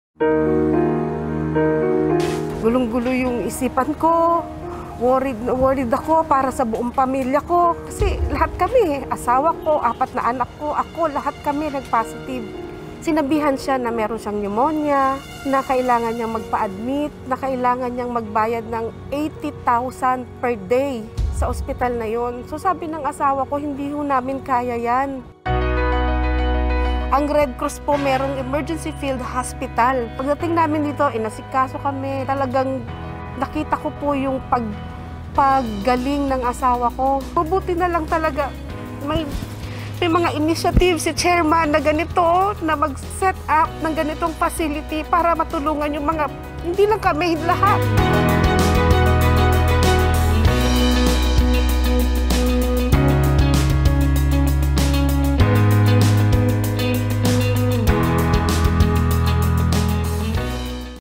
MAY PAG-ASA. Red Cross Emergency Field Hospital Tents
MAY PAG-ASA. Isa si Maria Victoria Manigan sa libu-libong pamilya na naging beneficiary ng Red Cross Emergency Field Hospital tents na itinayo natin sa iba’t ibang ospital sa buong bansa. Ito ang naging agarang aksyon natin upang masolusyonan ang problema sa kakulangan ng kama dahil sa patuloy na pagtaas ng kaso ng COVID-19.

Walang problema ang hindi malalampasan kapag may kusang-loob, malasakit, at kapwa-tao, at may pagkakaisa ng bawat Pilipino.